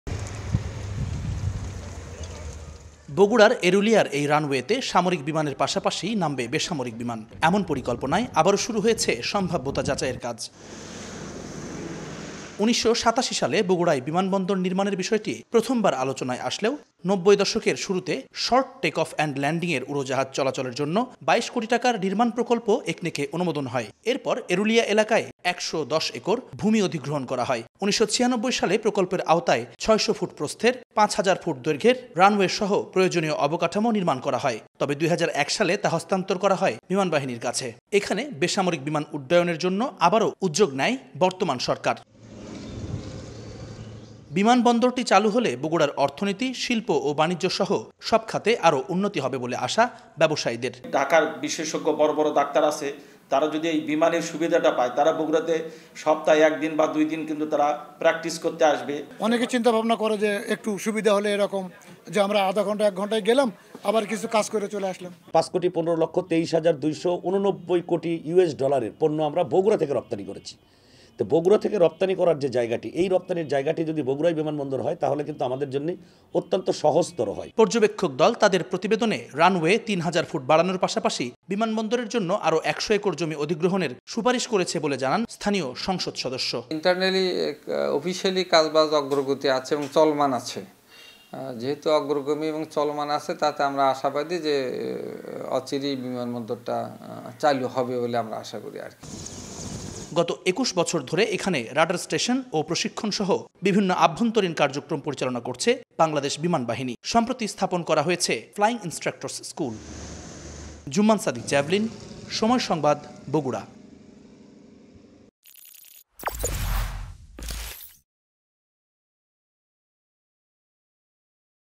পূর্ণাঙ্গ বিমানবন্দরে রূপ নিচ্ছে বগুড়ার ফ্লাইং ইন্সট্রাক্টরস স্কুল ! | Bogura Airport | Somoy TV
বগুড়ার এরুলিয়ার এই রানওয়েতে সামরিক বিমানের পাশাপাশি নামবে বেসামরিক বিমান। এমন পরিকল্পনায় আবারও শুরু হয়েছে সম্ভাব্যতা যাচাইয়ের কাজ। বিমানবন্দরটি নির্মাণ হলে বগুড়ার আর্থসামাজিক অবস্থা ব্যাপকভাবে পাল্টে যাবে বলে মনে করেন ব্যবসায়ীরা।